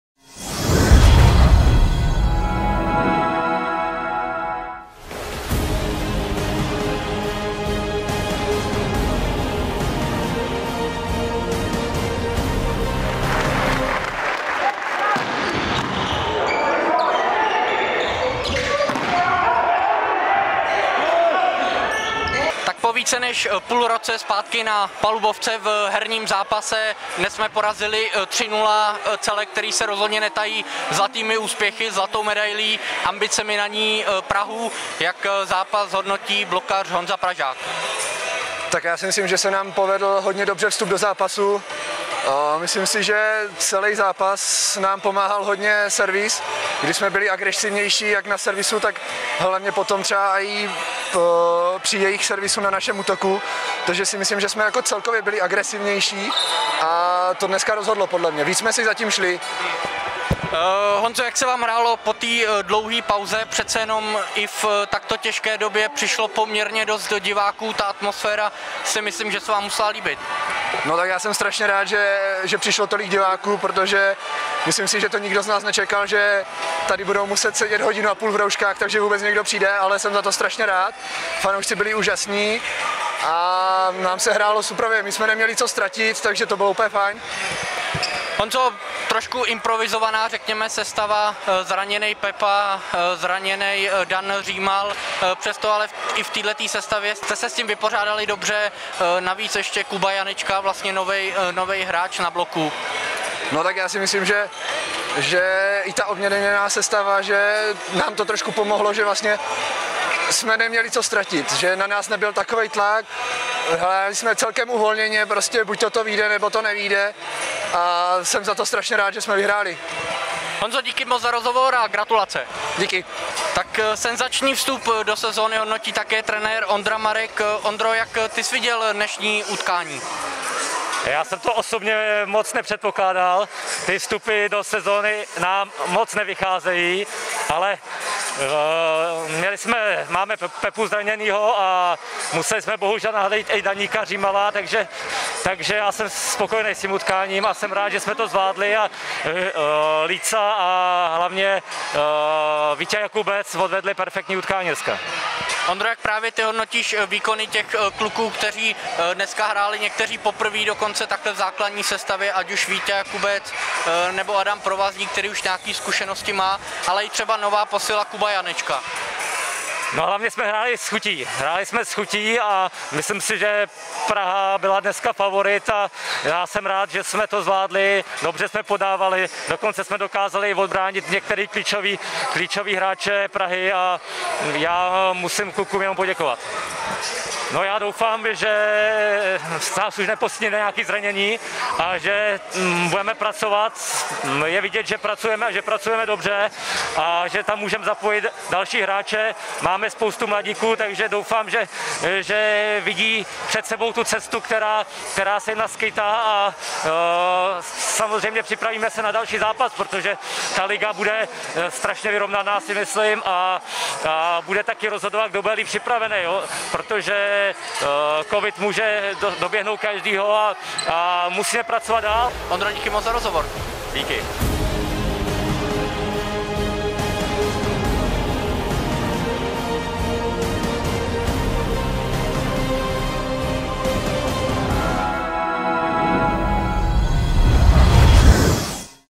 2021: Sestřih utkání Brno - Praha
1. kolo UNIQA extraligy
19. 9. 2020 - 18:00
Volejbal Brno - VK Lvi Praha 3:0 (22, 28, 28)